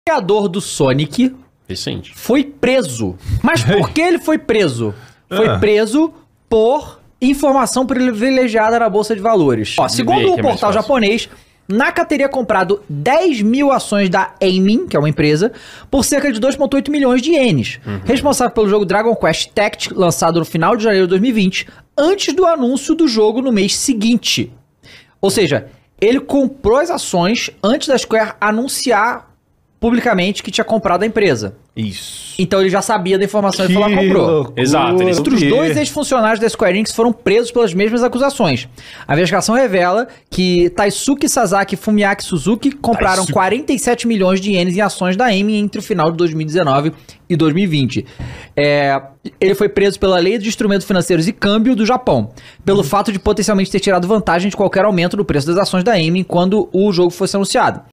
Criador do Sonic foi realmente preso?
Criador do Sonic "Yuji Naka" realmente foi pra cadeia?